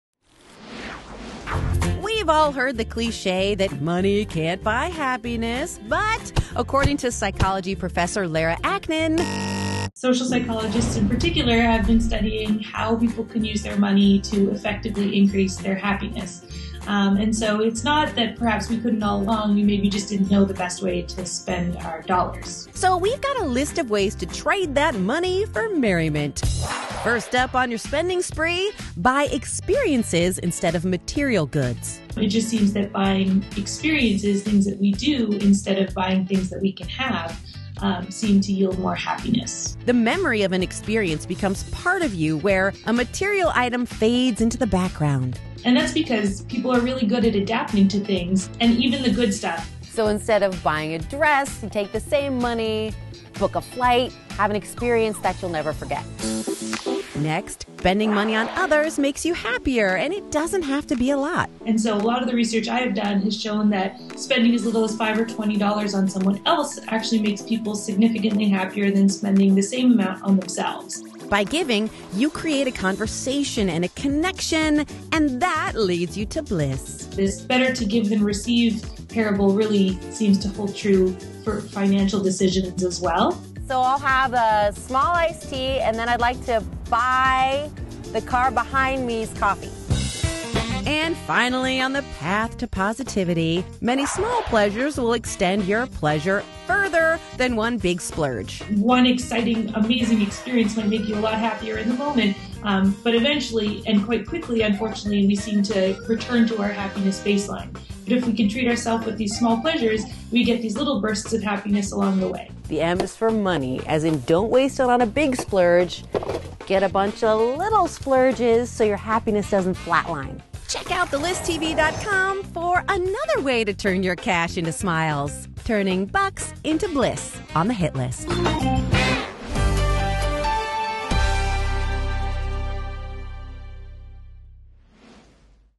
3 Ways Money CAN Buy You Love and Happiness
You've heard money can't buy love and happiness, but that's not completely true. You’ve just got to work the system. Teresa Strasser and Psychology Professor, Lara Aknin, have 3 ways how you can get the most happiness out of your bucks because money CAN actually buy you love!